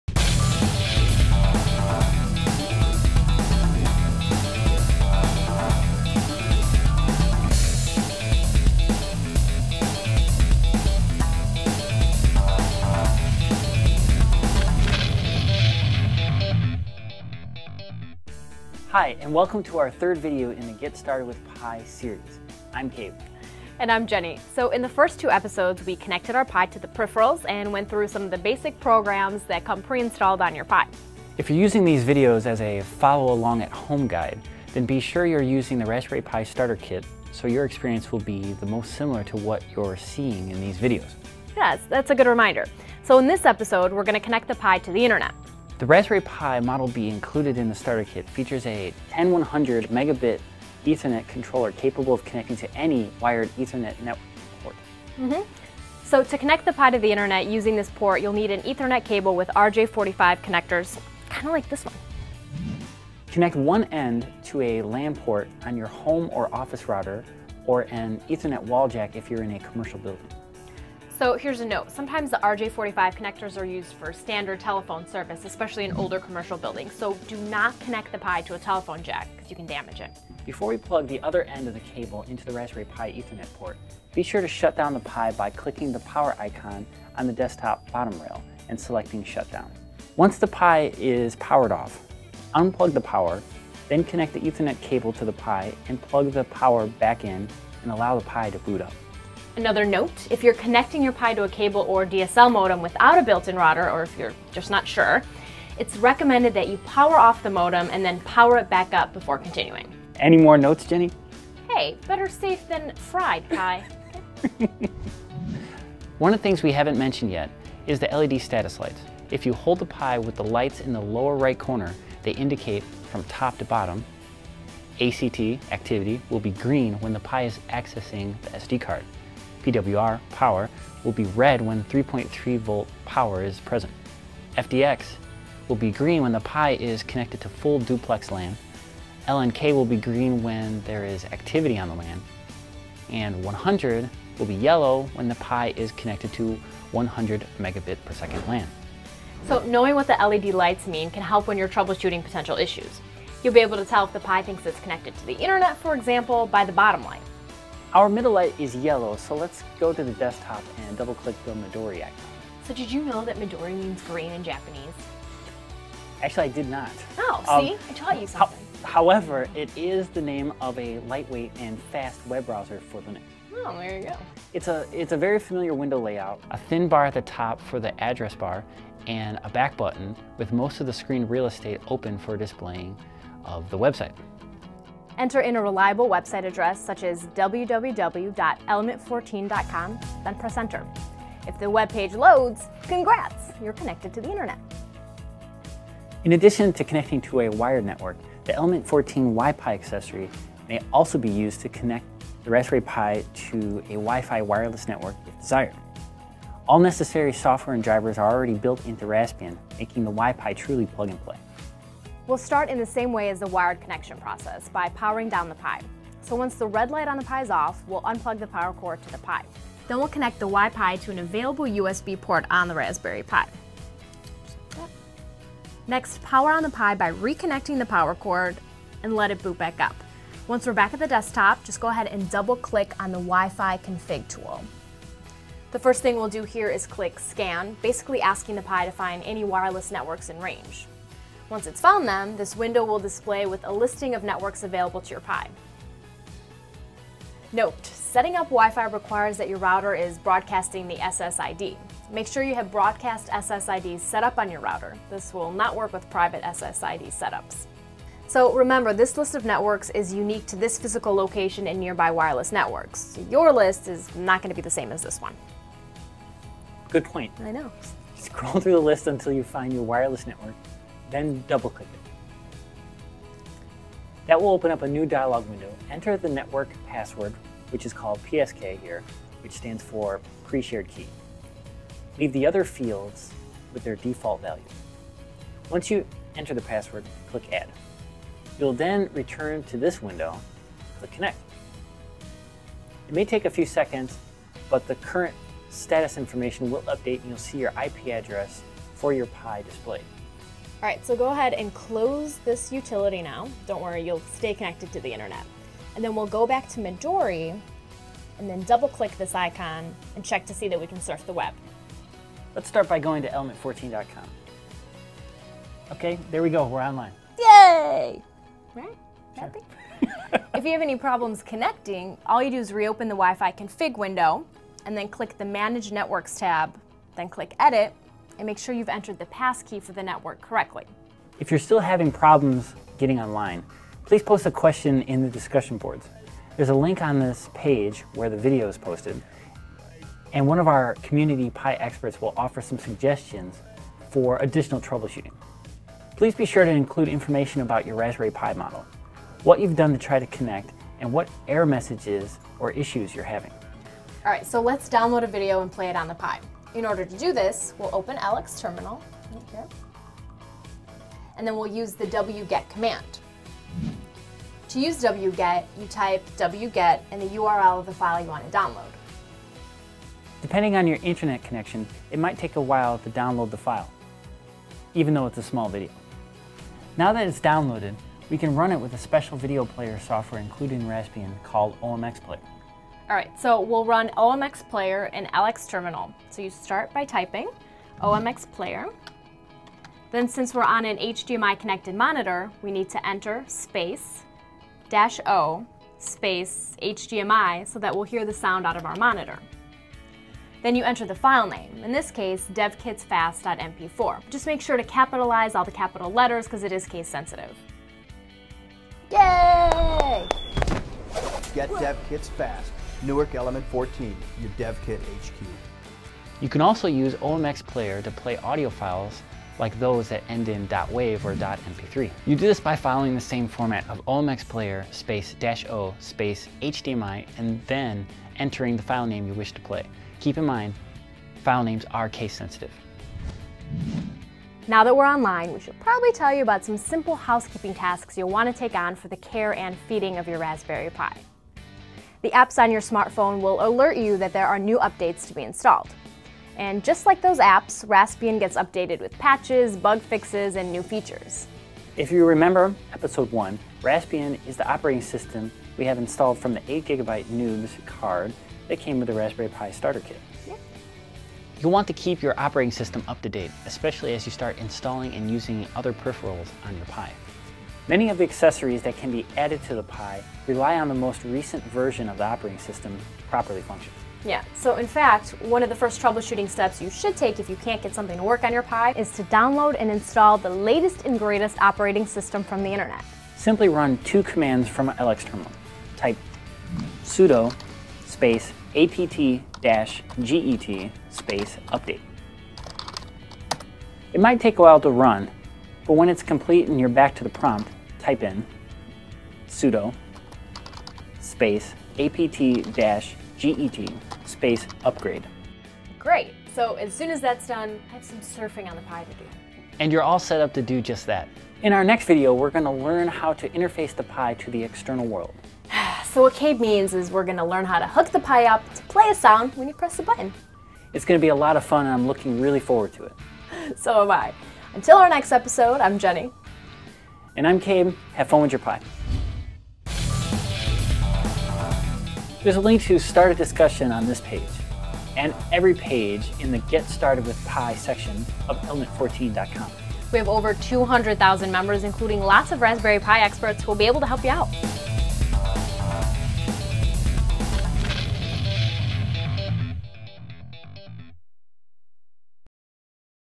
Get Started with Raspberry Pi 3 - Online with Pi
The third video in our five-part series on how you can Get Started with Raspberry Pi. for more information about getting started, accessories and projects you can build with your Pi.